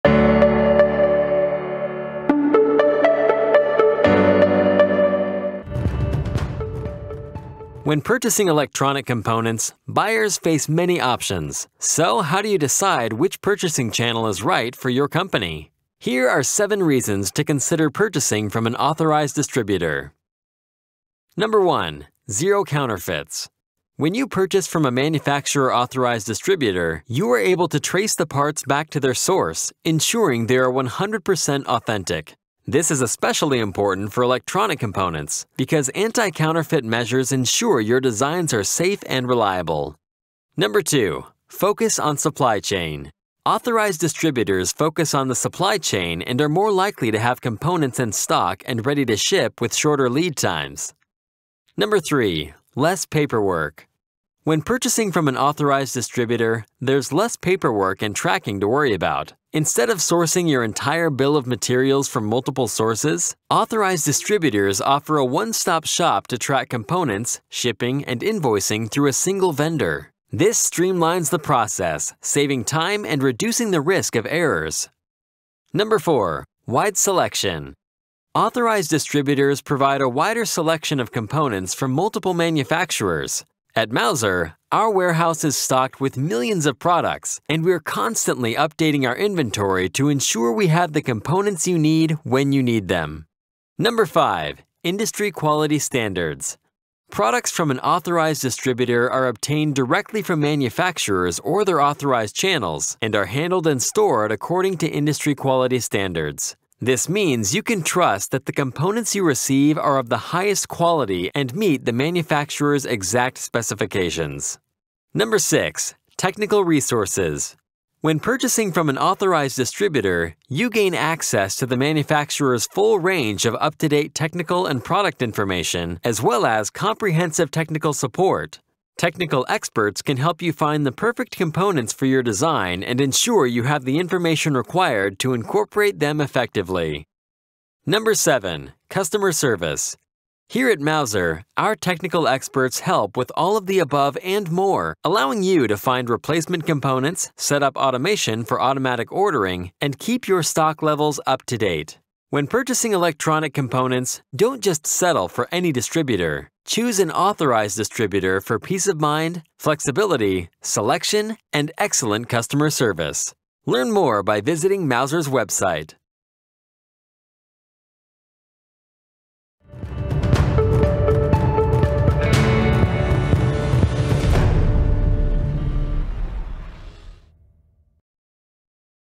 Off the Belt: 7 Reasons to Purchase from an Authorized Distributor | Mouser Electronics
Discover the benefits of choosing an authorized distributor for your electronic component purchases. From authenticity and streamlined supply chains to extensive selections and exceptional customer service, discover why an authorized distributor like Mouser is your ideal partner in procurement.